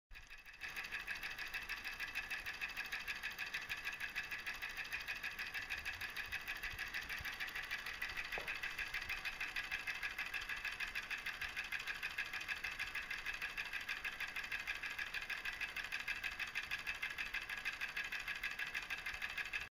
Meccano Erector Arduino twin cylinder with Electrikit parts
Meccano Electrikit motor two cylinders (solenoids) controlled by Arduino